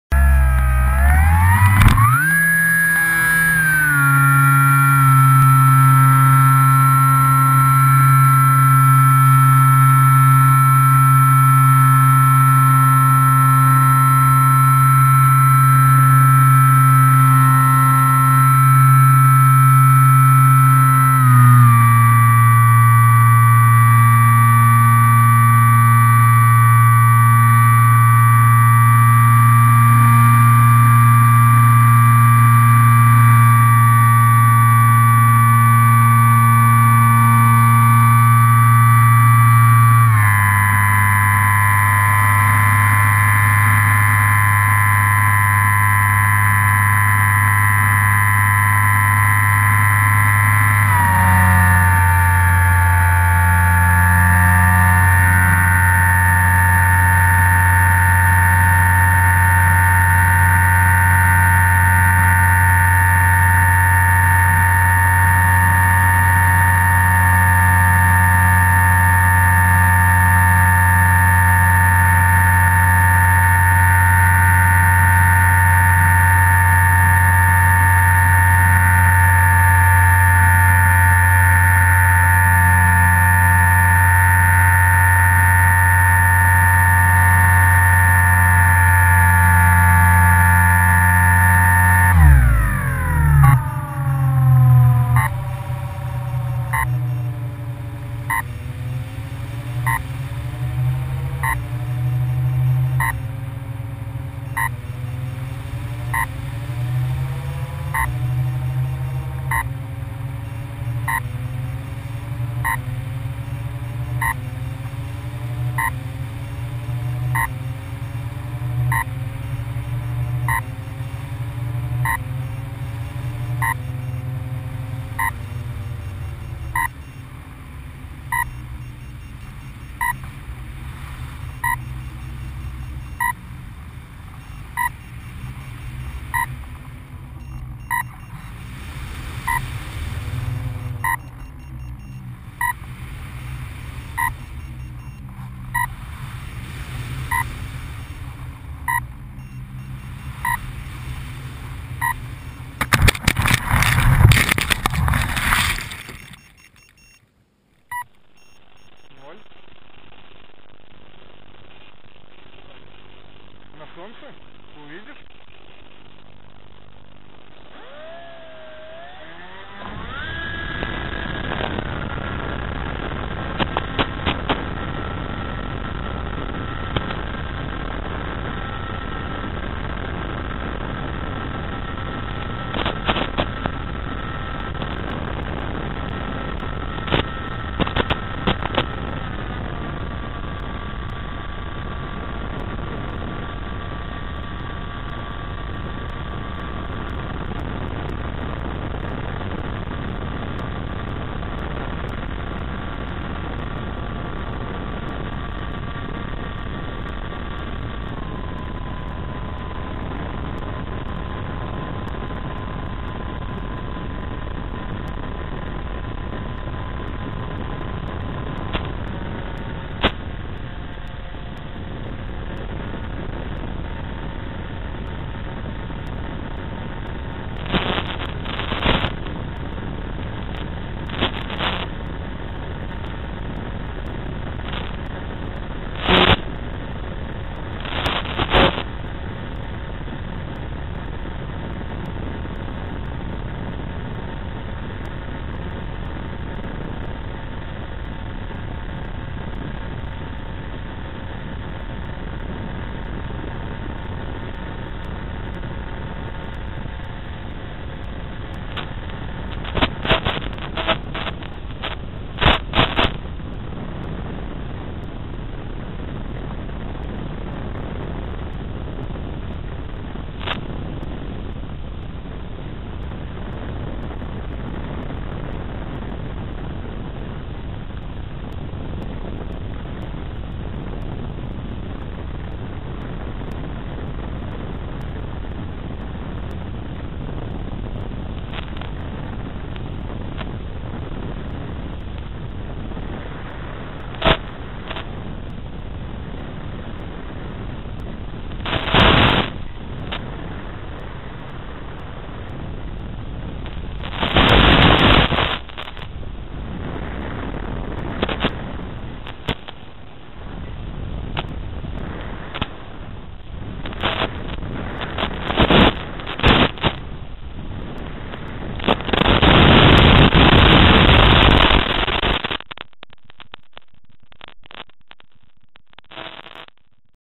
EPP-FPV First crash
My first crash on EPP FPV due RC Tx battery dead. Plane lost signal (when the motor beeps) and independently glide to ground. There are no damages at all. This is my the most successful crash!!!
 Video is from GoPro2 and Remzibi's OSD
 EPP FPV 1.8m
Li-Po Nano tech 6000mAh; 60A ESC; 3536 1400kv; APC 11x5.5 Remzibi OSD; GoPro Hero 2; Video Tx Rx 5.8 GHz 200mW; Cloveleaf antenna; Turnigy 9X V2; 2.4 Ghz 2W booster.